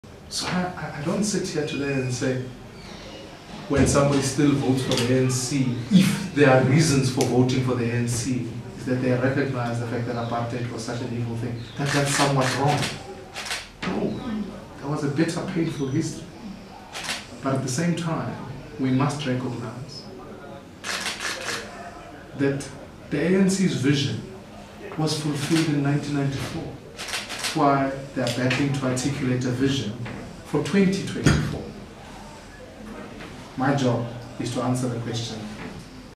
Copy of Interview with Da Leader Mmusi Miamane
DA leader, Mmusi Miamane visitd Polokwane today as part of his national Freedom, Fairness & Opportunity Tour.